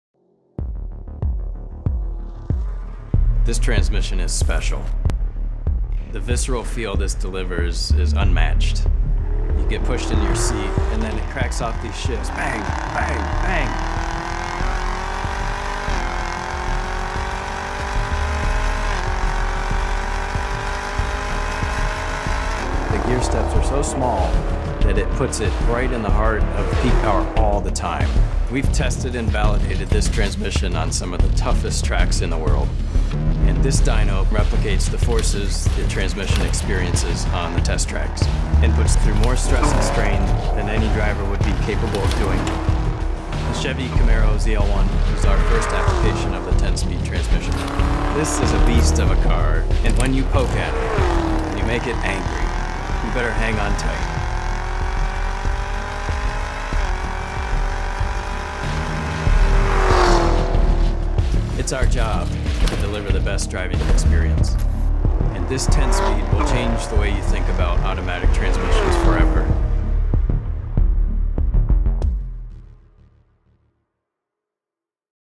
2017 Camaro ZL1 – Redefiniendo la Transmisión Automática
Chevrolet nos presenta la nueva transmisión autimática de 10 relaciones y que se estará estrenando en el nuevo Chevy Camaro ZL1 2017.